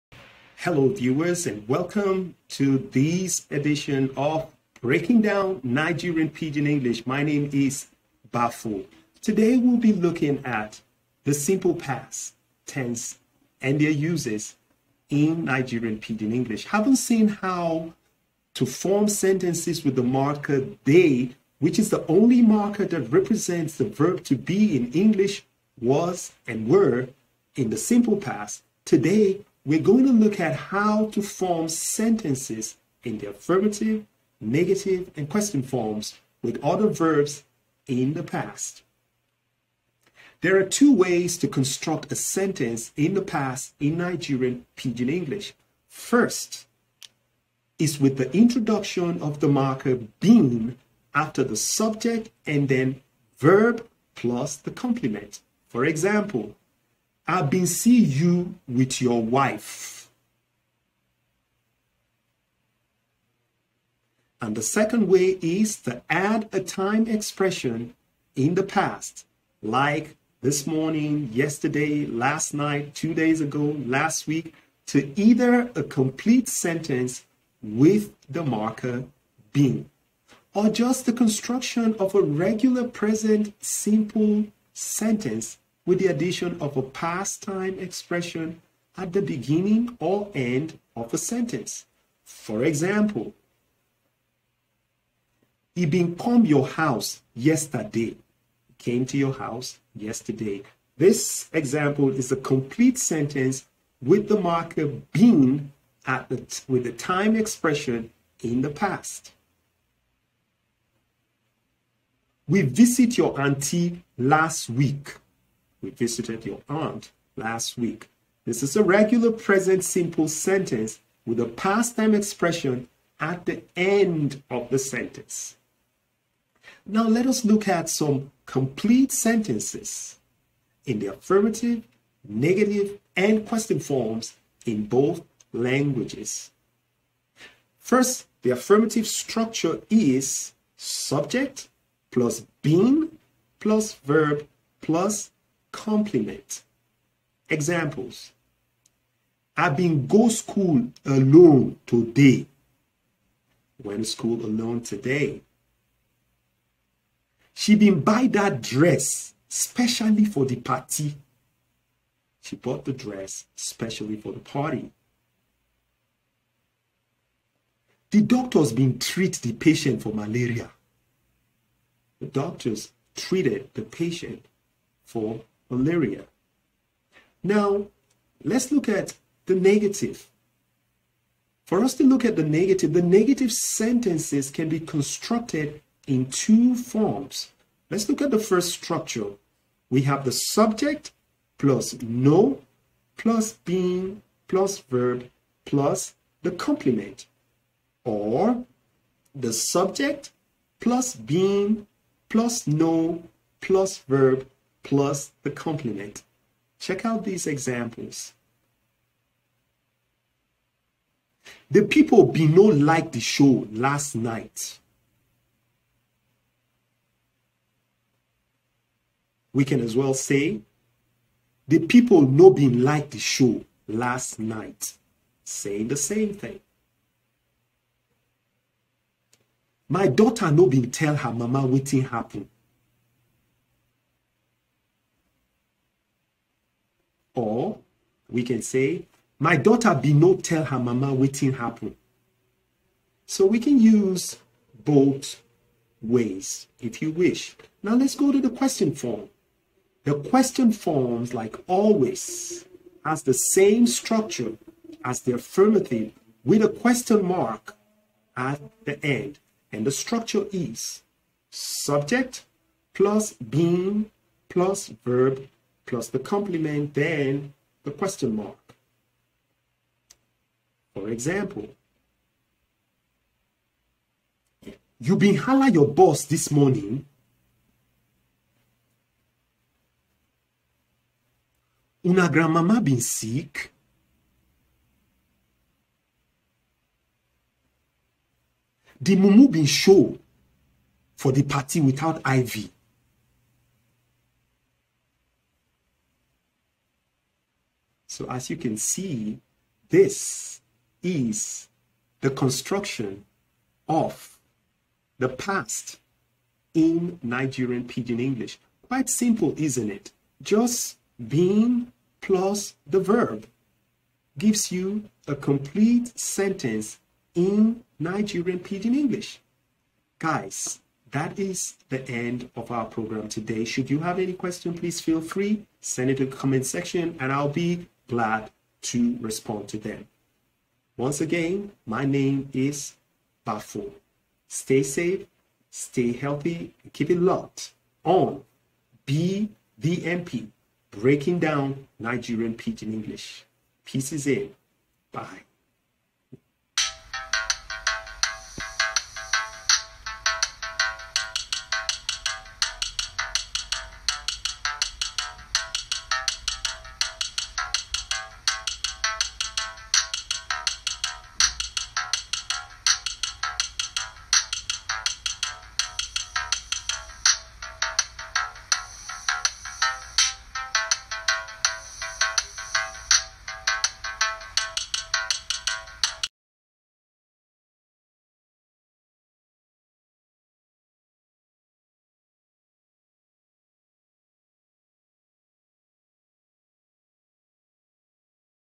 NIGERIAN PIDGIN - LESSON 12 - USES OF SIMPLE PAST
Having looked at the present simple, and the future tenses, the Simple Past is inevitable.

Knowing the uses of the grammar rules and most topics on this channel will help viewers especially non-Nigerians understand Nollywood movies and Nigerian Afrobeat music better.